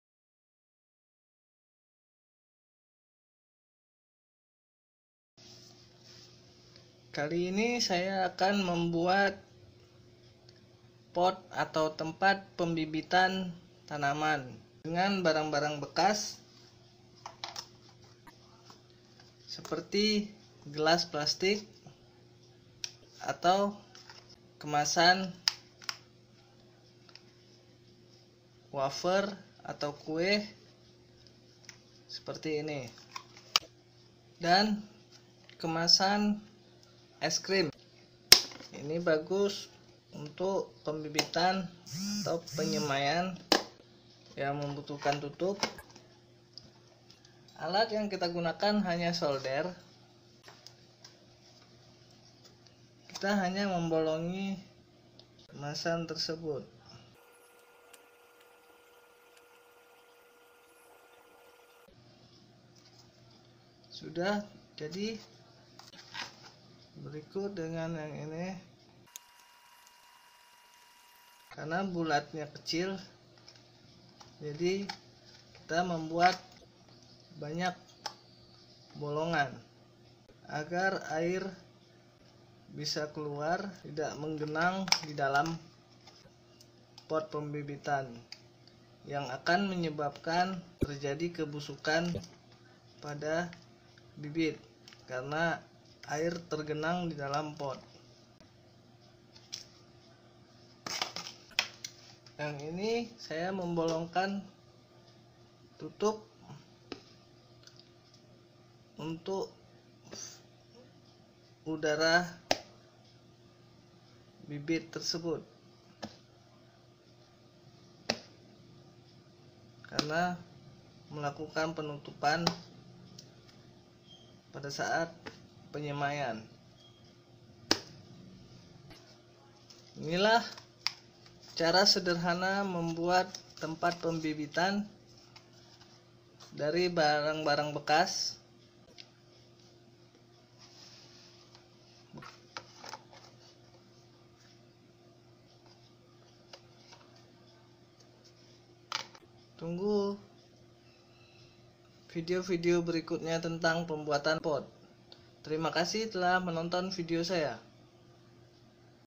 Cara Mudah Buat Pot Pembibitan Tanaman Dari Barang Bekas
dalam video ini menjelaskan cara membuat pot pembibitan dari abrang bekas